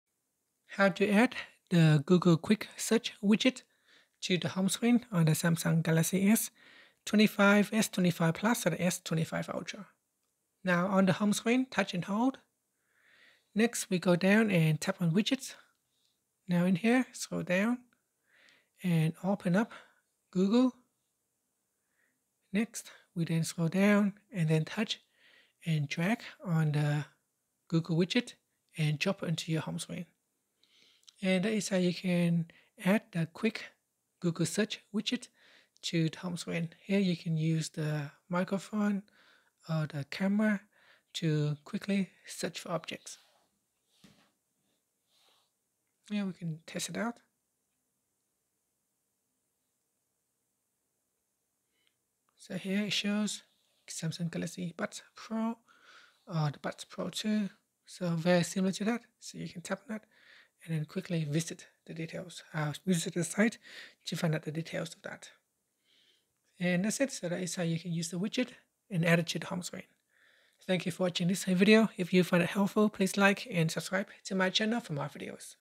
🔍 How to Add Google Search Widget on Galaxy S25 Home Screen (FAST & EASY!)
Want instant Google access from your home screen? In this quick tutorial, you’ll learn how to add the Google Quick Search widget to your Samsung Galaxy S25/S25+/Ultra. Whether you want faster searches or a cleaner layout, this simple step-by-step guide shows you how to personalize your device and boost your daily productivity in seconds!

🛠️ Step-by-Step Instructions:
Step 1: Long press on any empty area of your home screen
Step 2: Tap on Widgets from the bottom menu
Step 3: Scroll down and find the Google section
Step 4: Tap on Google, then press and hold the "Search" widget
Step 5: Drag it to your desired spot on the home screen and release
Step 6: Done! Google Quick Search is now on your home screen for instant access

📌 This video also covers topics:
✅ How to customize widgets on Galaxy S25
✅ Add Google bar to Samsung S25 home screen
✅ Improve search speed on Galaxy S25 with one tap access